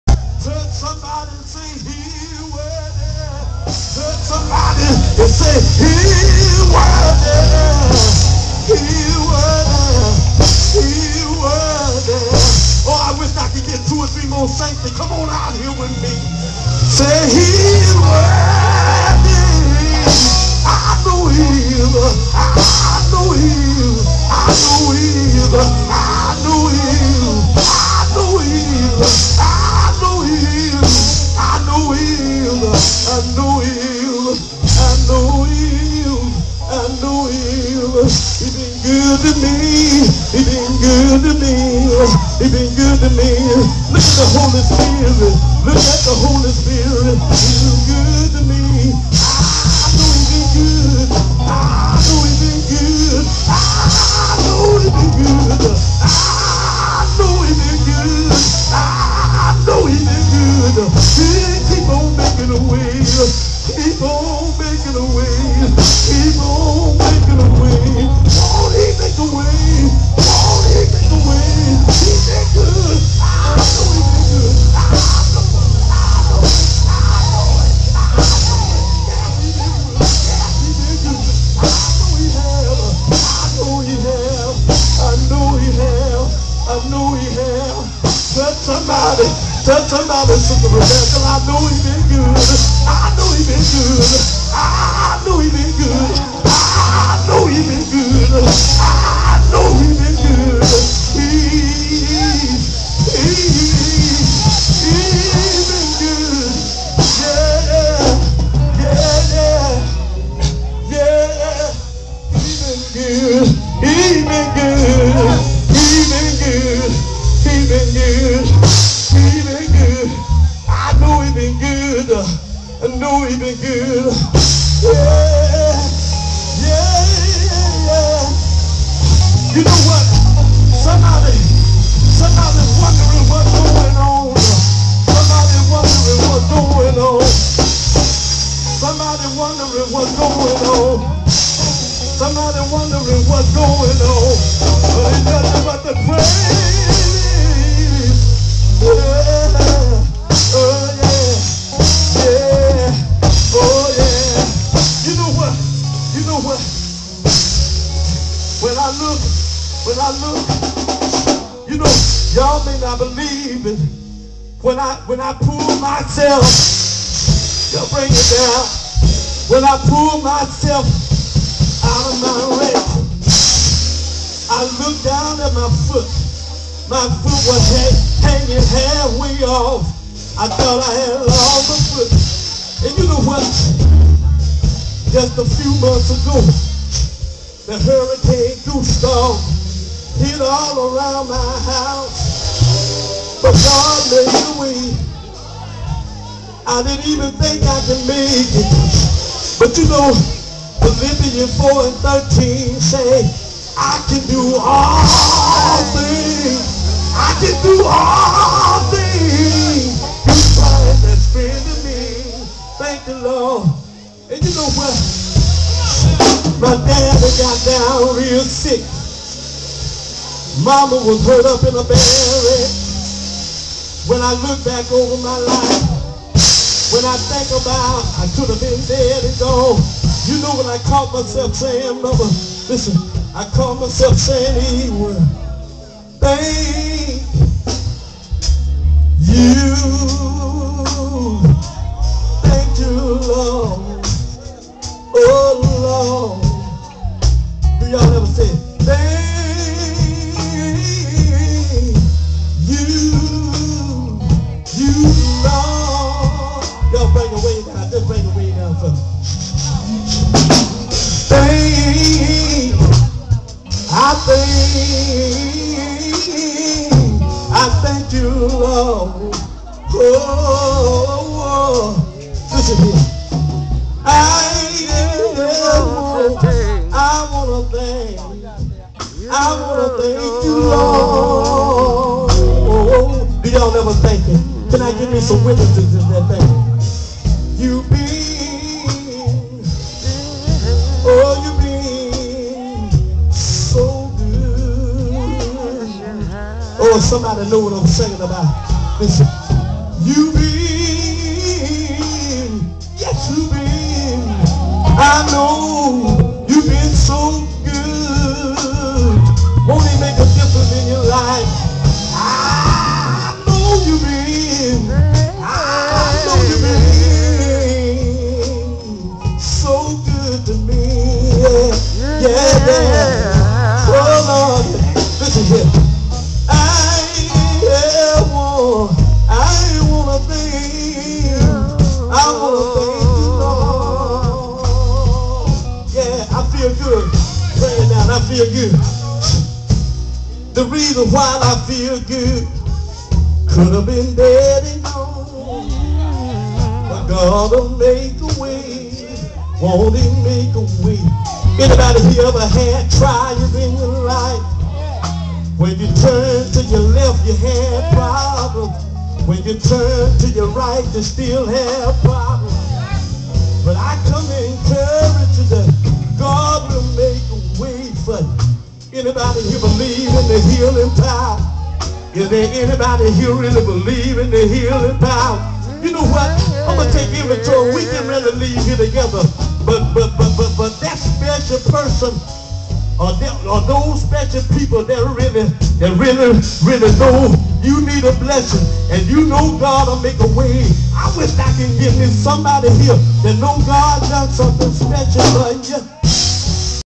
The Veal Brother of Gloster Mississippi-11-23-08
Look at those women gettin their praise on!!! God has been good!!! We wanna say "THANK U LORD"!!!